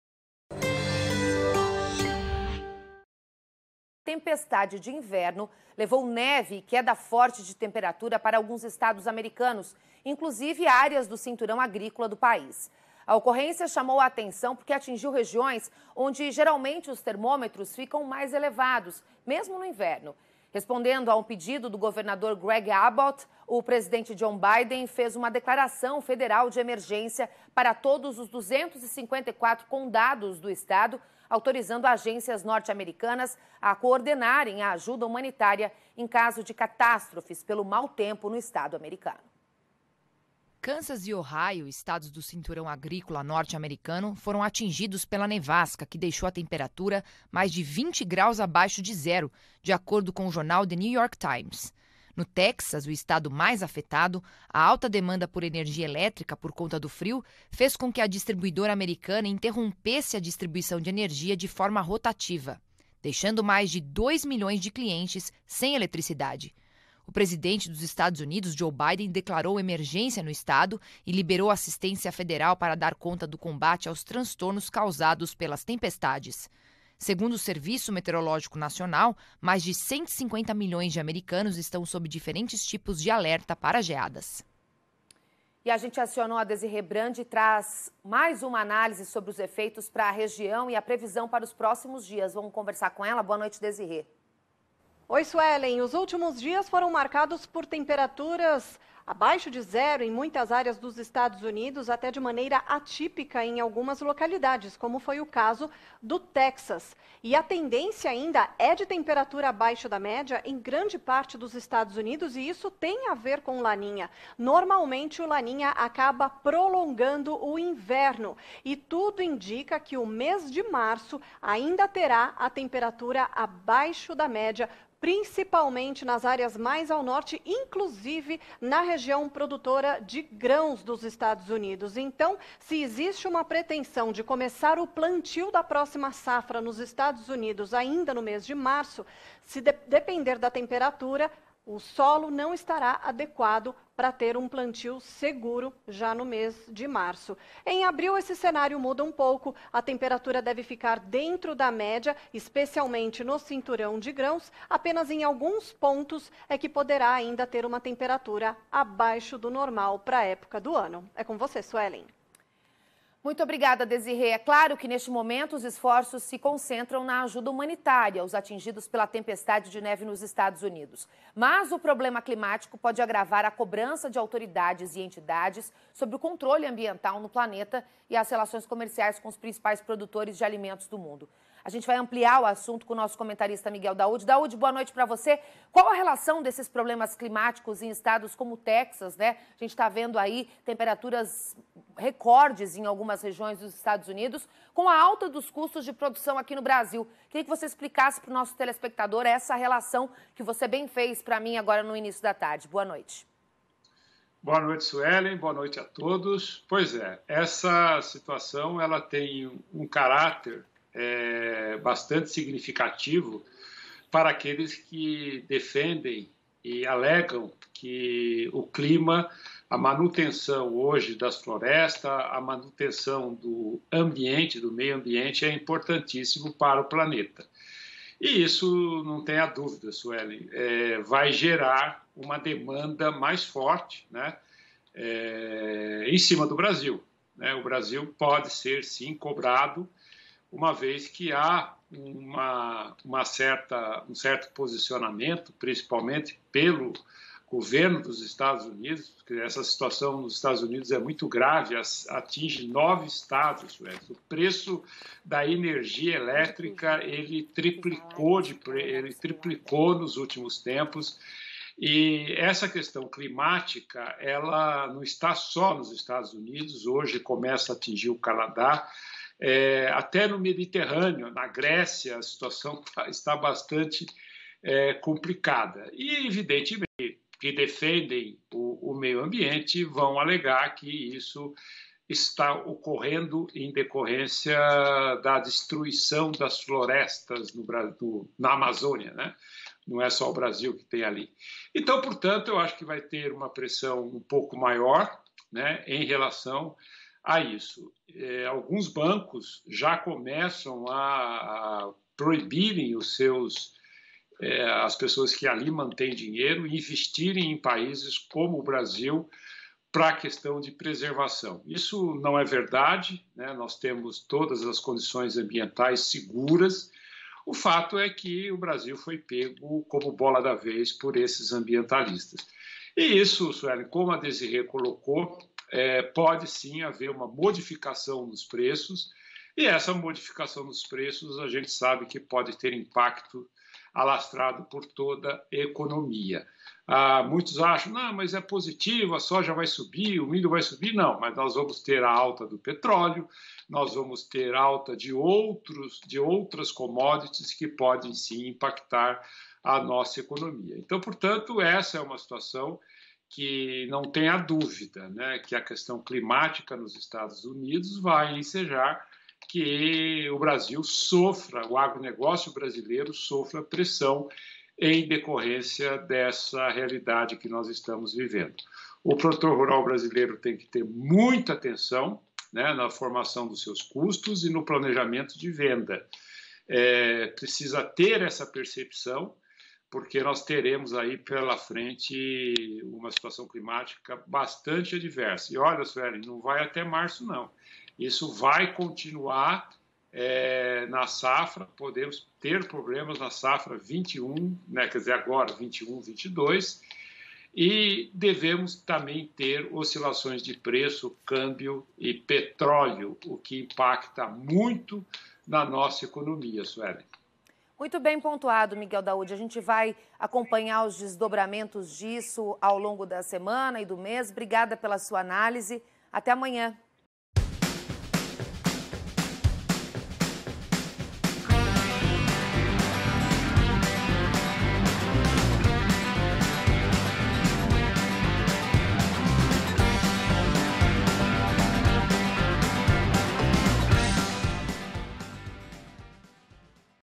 Daoud: Nevasca nos EUA pode aumentar tensão sobre questão ambiental
Uma tempestade de inverno levou neve e queda forte de temperatura para alguns estados americanos, inclusive áreas do cinturão agrícolas do país. A ocorrência chamou a atenção, porque atingiu regiões onde, geralmente, os termômetros ficam mais elevados, mesmo no inverno.

Kansas e Ohio, estados produtores de milho, soja e trigo foram atingidos pela nevasca que deixou a temperatura mais de 20º C abaixo de zero, de acordo com o jornal The New York Times. No Texas, o estado mais afetado, a alta demanda por energia elétrica devido ao frio, obrigou a distribuidora norte-americana a impor blecautes rotativos que deixaram mais de 2 milhões de clientes sem eletricidade nesta segunda-feira, 15.

Segundo o comentarista do Canal Rural a questão envolvendo problemas climáticos pode agravar a cobrança de autoridade e entidade sobre o controle ambiental no planeta e as relações comerciais com os principais produtores de alimentos do mundo. “Essa situação tem um caráter bastante significativo para aqueles que alegam que a manutenção das florestas e do meio ambiente não são importantes para o planeta. Isso vai gerar uma demanda mais forte para cima do Brasil e o nosso país poderá, sim, ser cobrado, uma vez que há um certo posicionamento principalmente pelo governo dos EUA. É uma situação muito grave essa do clima e algumas correntes culpam o desmatamento na região da Amazônia”, explicou.

“Alguns bancos já começam a proibir pessoas que ali mantêm dinheiro de investir dinheiro no Brasil. Ou seja,  o nosso país foi pego como bola da vez pelos ambientalistas. Isso pode gerar modificações nos preços e pode ter impacto alastrado por toda a economia, pois subirão as commodities agrícolas, mas outras como o petróleo também vão impactar a atividade”, concluiu.

Assista também pela sua TV:
NET: 185 e 685
Sky: 164
OiTV: 179
ClaroTV: 185
Parabólica Analógica: banda C na Star One C2
Parabólica Digital: Star One C2